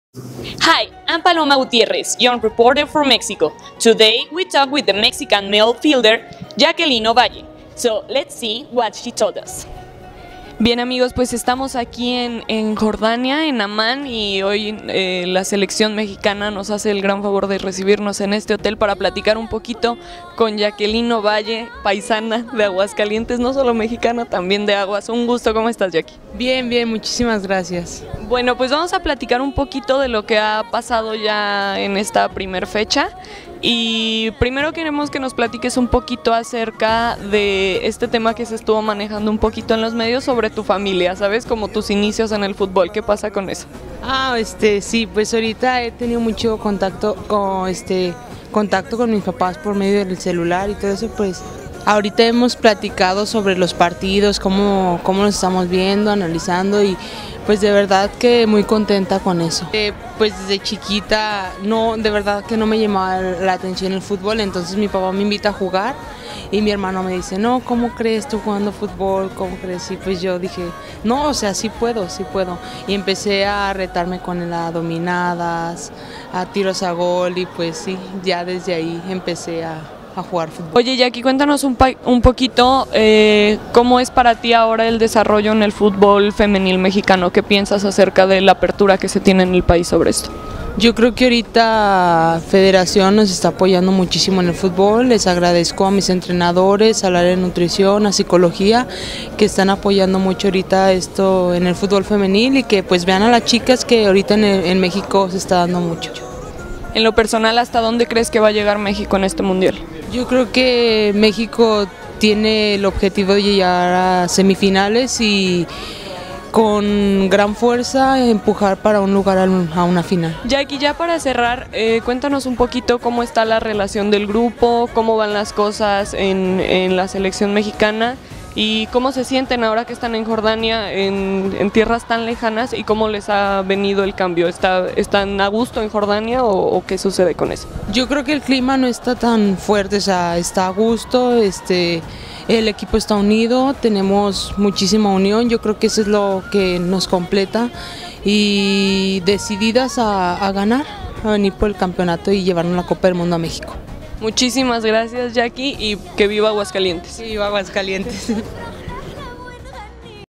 AIPS Young Reporters @ FIFA U17WWC: interview with Jacqueline Ovalle, Mexico forward
By AIPS Young Reporter Paloma Gutierrez Mendez